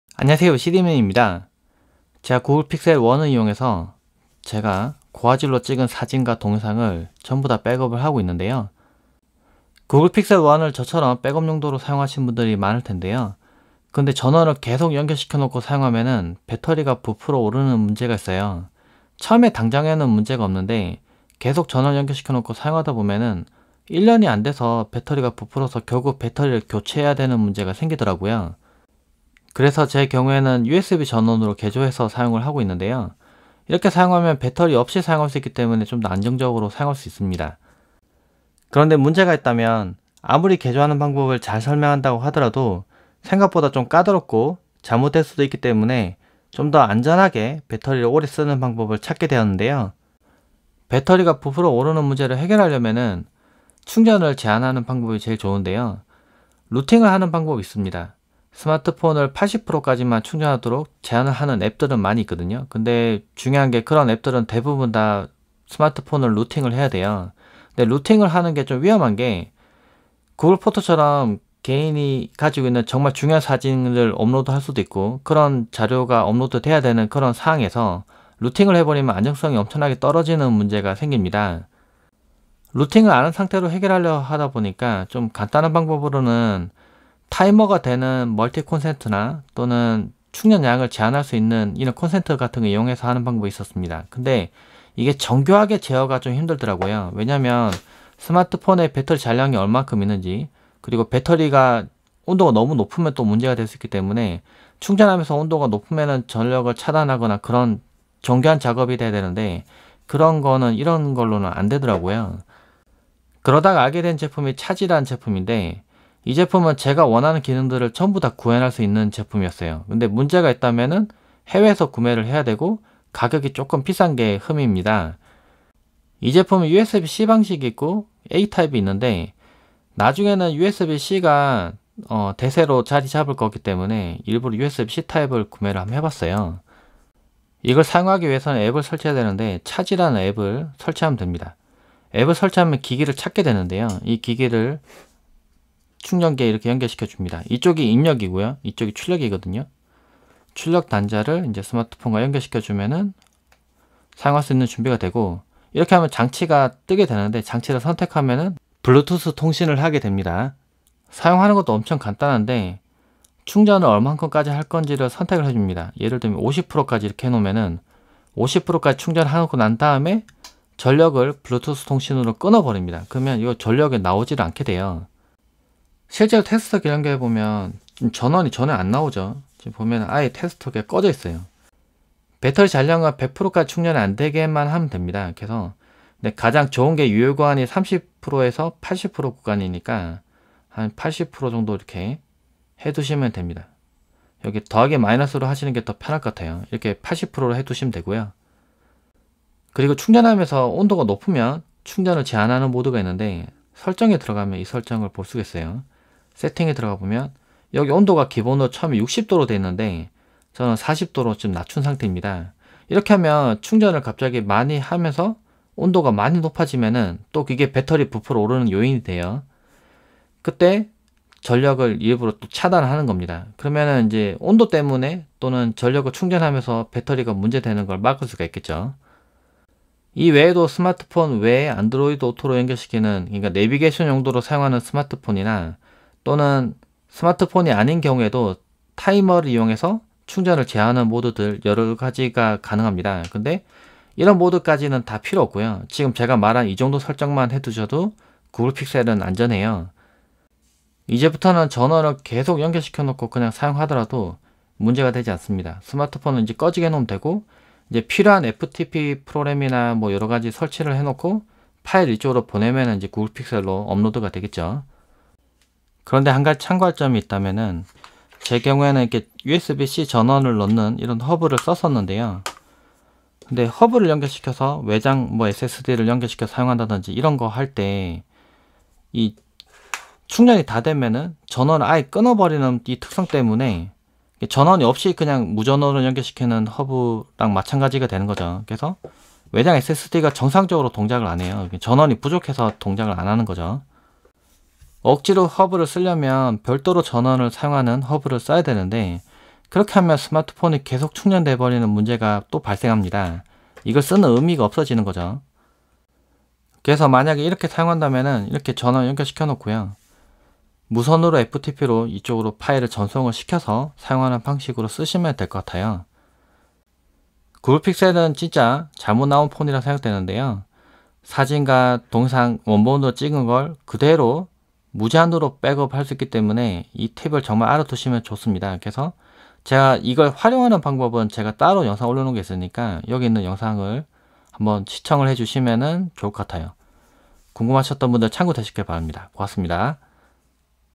루팅 안하고 구글픽셀 배터리 충전 80% 제한해서 배터리 수명 연장하는 방법 chargie
구글픽셀 이용해서 구글포토로 사진, 동영상 원본을 잘 저장하고 있는데요. 구글픽셀은 계속 충전시 배터리가 부풀어 올라 고장나는 문제 증상이 있습니다. 매번 배터리를 교체하는 것도 부담스러운데요. USB 전원으로 개조하는 방법이 있지만, 교체 방법이 좀 복잡하고 따라하기에는 초보자는 좀 어려움이 있습니다. 분해하거나 개조하는 과정 중 구글픽셀이 고장나는 문제도 발생할 수 있습니다. 그래서 좀 더 안전하게 구글픽셀1을 배터리를 충전을 제한하는 방법을 찾게 되었는데요. 루팅을 하면 간단하게 해결이 되지만, 민감하고 중요한 사진, 동영상을 백업하기 위한 목적으로 쓰는 폰을 루팅을 하면 안정성(보안성)이 많이 떨어지는 문제가 생깁니다. 루팅을 안하고 충전을 제한하려고 여러가지 방법을 찾아봤는데요. chargie 라는 기기를 이용하면 간단하게 해결이 가능했습니다. chargie는 스마트폰 앱과 블루투스 통신을 하면서 배터리 잔량과 배터리 발열 상황등을 불러와서 충전을 하거나 또는 충전을 멈출 수 있습니다. 사용자가 임의로 배터리 잔량을 설정할 수 있으며, 80%로 설정 시 배터리가 부족하면 충전하고 배터리가 80%에 도달하면 배터리 충전을 멈춥니다. 계속 이 과정을 반복하면서 배터리 스웰링이 발생하는 것을 방지 합니다. 원래는 배터리 100% 충전한다고 해서 배터리가 부풀어오르는 문제가 발생하면 안되지만, 구글픽셀3도 배터리가 부풀어 오르는 문제가 발생한다고 하죠. 배터리 성능이 좋지 않은 것 같긴 합니다. 다만 chargie를 이용하면 배터리 충전을 강제로 제한을 해서 배터리를 더 오래 쓸 수 있습니다. 구글픽셀은 원래 사진, 동영상을 백업할 목적으로만 쓰기 때문에 충전을 계속 시켜도 문제가 안되는데요. 이렇게 활용하면 오랜 시간 백업서버로 활용이 가능할 것 같습니다.

00:00 구글픽셀 필요성
00:09 구글픽셀 배터리 문제
00:28 USB 전원 개조 위험성
00:50 구글픽셀 충전 제한 방법들
01:52 chargie 이용해서 충전 제한
04:25 USB 허브 사용 제한
05:16 마무리

촬영장비 : Sony A7M4 , Sony FDR-AX100 , INSTA360 One X2 , Mavic Pro, 고프로9, 오즈모액션, Sony ZV-1
편집도구 : 파워디렉터 18 , 프리미어프로